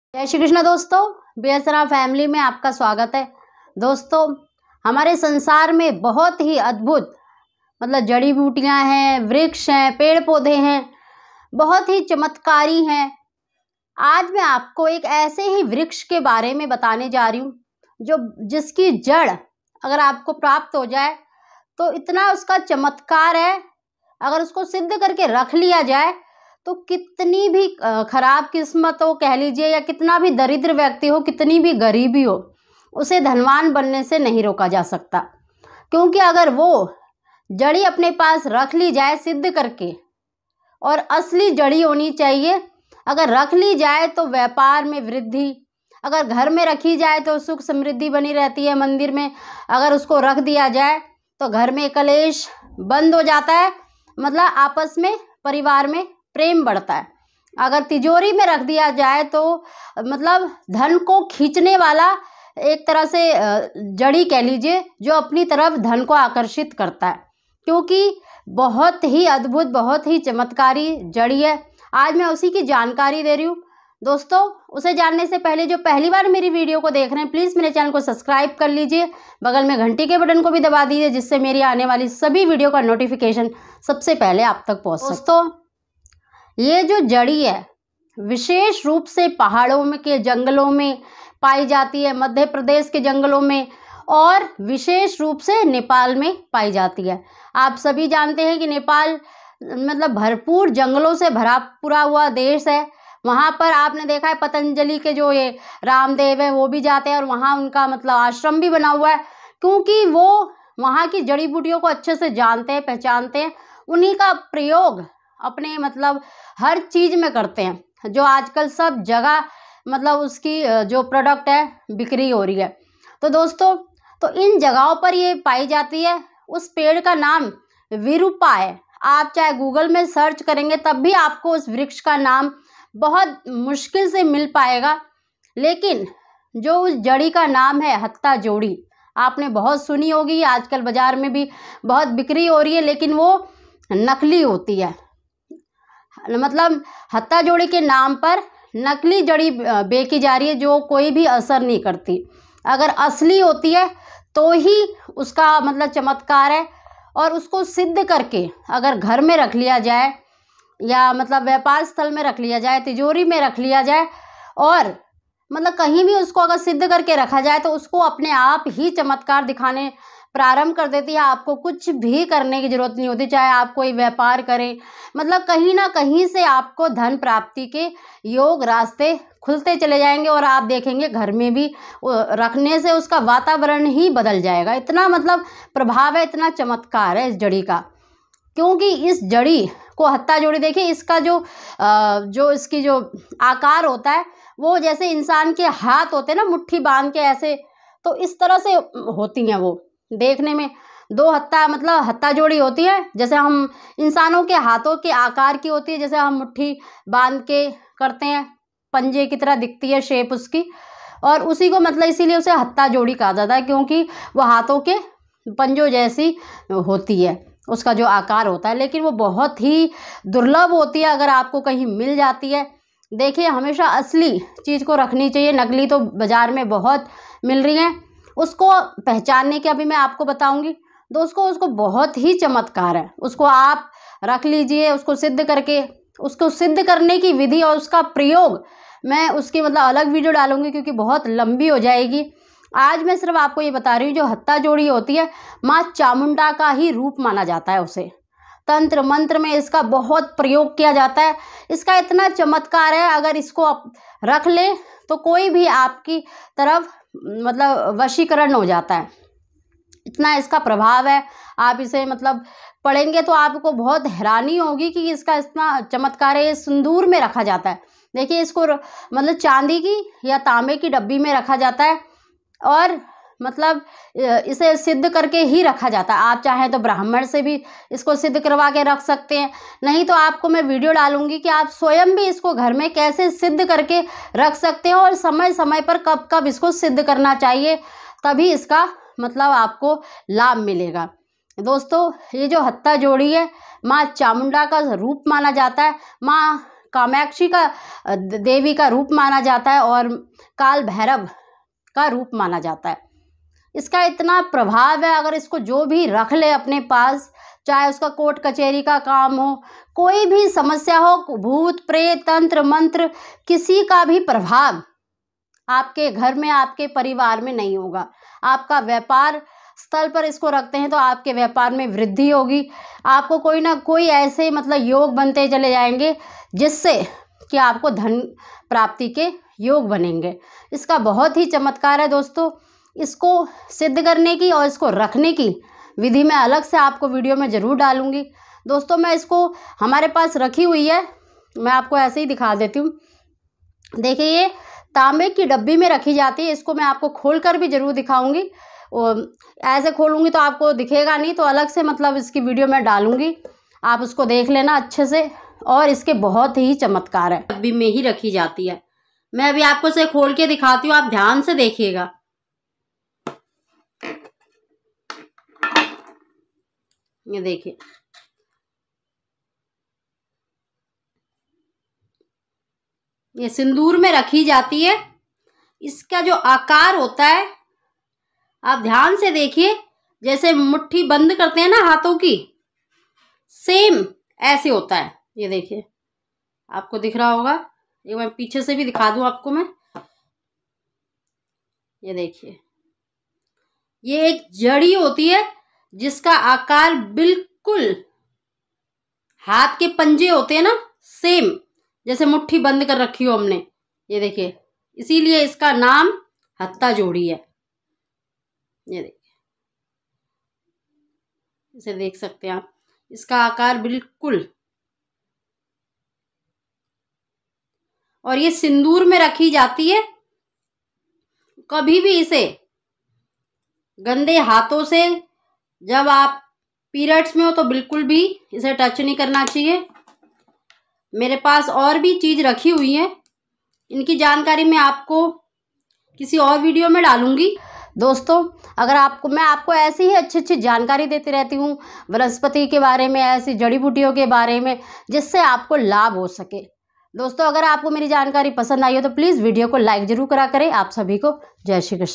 हत्था जोड़ी को सिद्ध करके घर में रखने से व्यक्ति को अमीर बनने से कोई नहीं रोक सकता। चमत्कारी जड़।
मदार की जड़ का प्रयोग गर्भाशय की बंद ट्यूब खोलने के लिए रामबाण इलाज का वीडियो लिंक